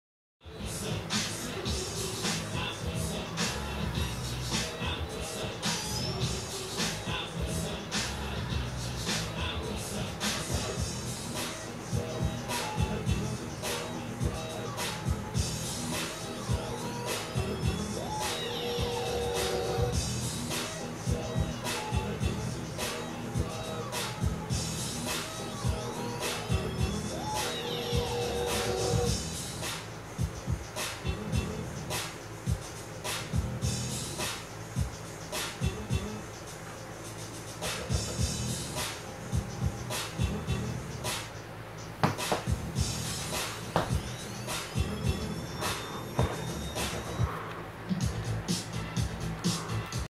Electrick Boogie Man "Freeze & transitions" 2015
Enchainement de positions en equilibre avec un controle sur les transitions Par : Electrick Boogie Man